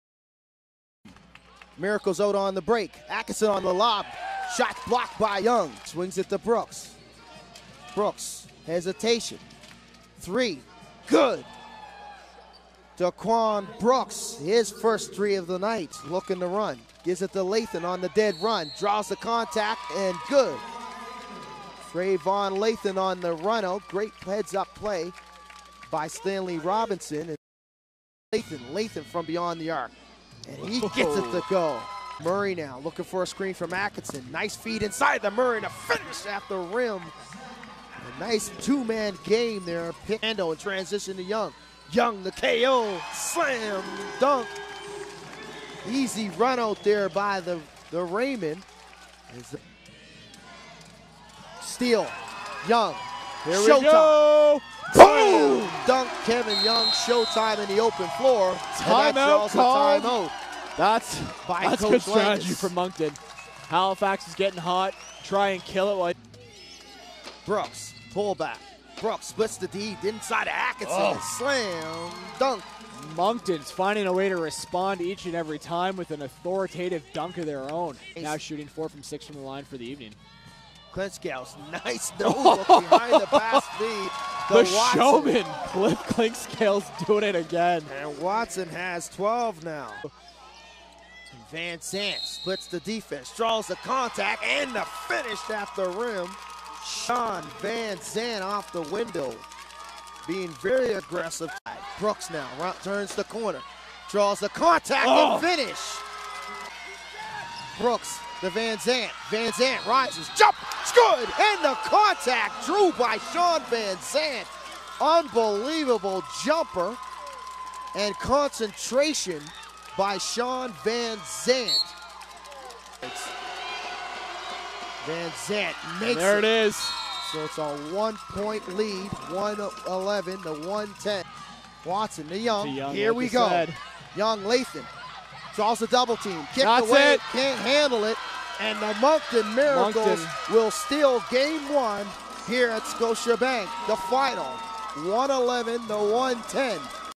Moncton Miracles Stun Halifax Rainmen in Opening Playoff Game!
For three quarters the Halifax Rainmen played like a team that had won 10 straight games to finish the regular season, but in the fourth quarter it all collapsed around them as the Moncton Miracles roared back to win game one of the best of five series 111 to 110.

Halifax looked to get off to a quick start, but Moncton was able to keep the contest close thanks to pin-point shooting from beyond the arc through the first 12 minutes of play. The second quarter saw the game remain close, with Moncton continuing their hot three point shooting, until the final two minutes of play in the half when Kevin Young took over and helped create a 14 point seperation heading into the third quarter. With Halifax holding the 61-47 lead, and winning important battles at the free throw line and under the rim, they looked poised to run away with the game. While Moncton continued to be a tough challenge Halifax found away to increase their lead in the third quarter and took an 18 point, 91-73, lead into the final 12 minutes.

Moncton came out strong in the fourth quarter and never let up, capitalizing on Halifax's sloppy play, hurting them with second chance opportunities and continuing hot shooting from three point land. Moncton finally took the lead in the final moments of the game, shutting down the Rainmen offense as they tried to make a final push.

Kevin Young had a double double for the Rainmen with 24 points and 10 rebounds. Liam McMorrow contributed 24 as well, and Tyrone Watson earned 18 points off the bench. The Moncton Miracles were led by Shawn Vanzant who had an impressive 28 points. Daquan Brooks had 25 points and Trayvon Lathan put away 22. Miracles rebounders were led by Shamus Ferguson who had 7 on the night.